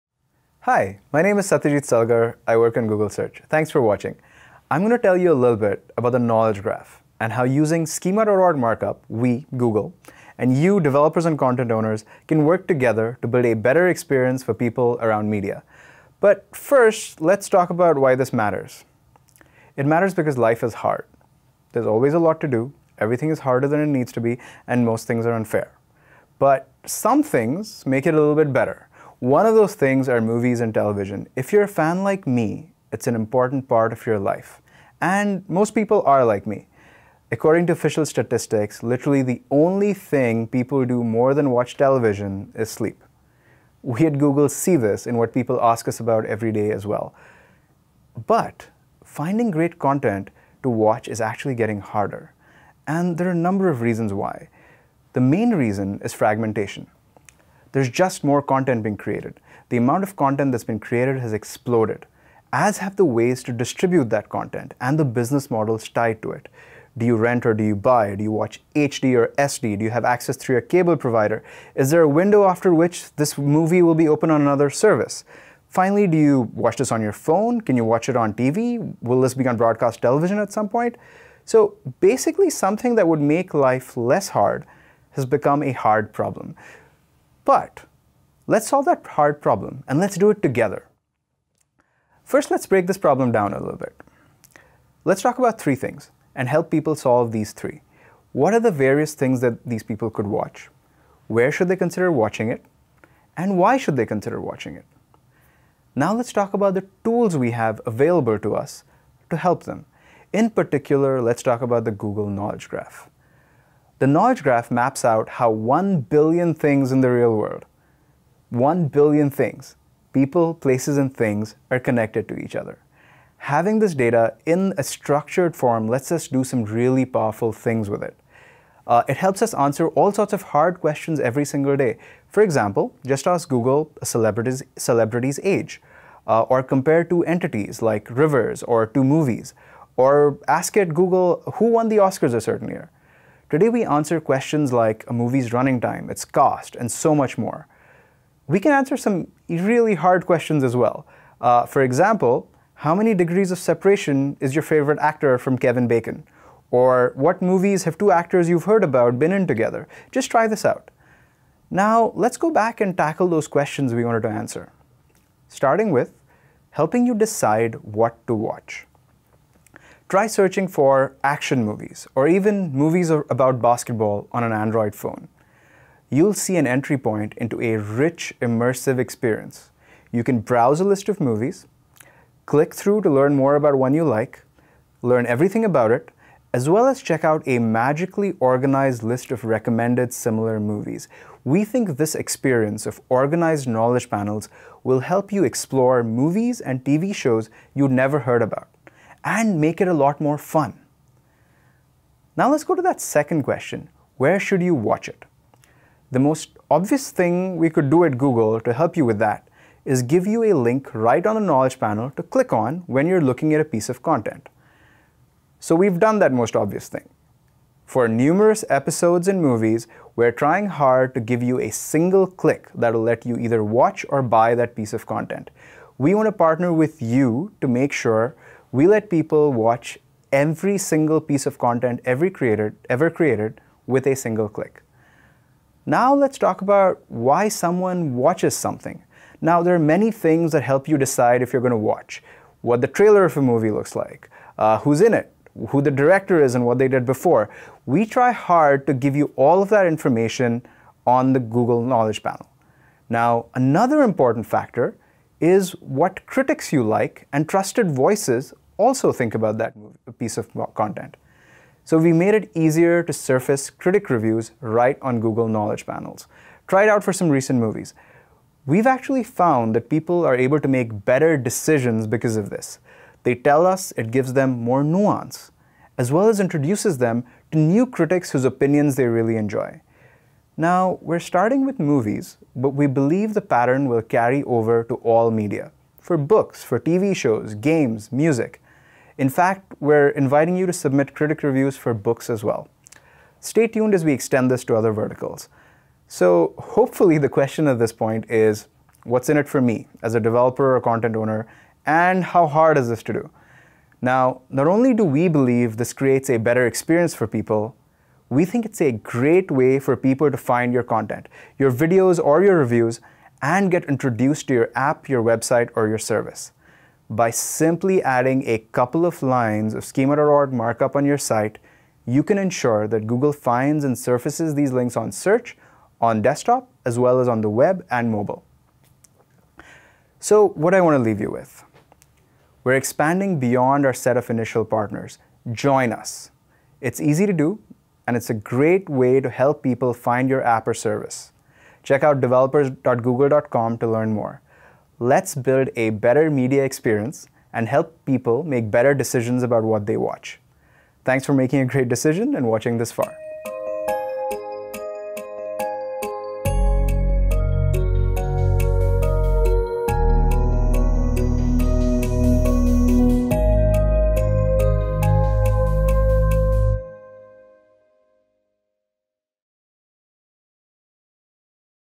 The Knowledge Graph and You: Video actions and movie reviews (100 Days of Google Dev)
The Knowledge Graph is an incredibly powerful tool and can help people make better decisions about the media they consume. Learn how your critic review and video actions can be surfaced on Google to help users find your content.

For more information about integrating with the Knowledge Graph,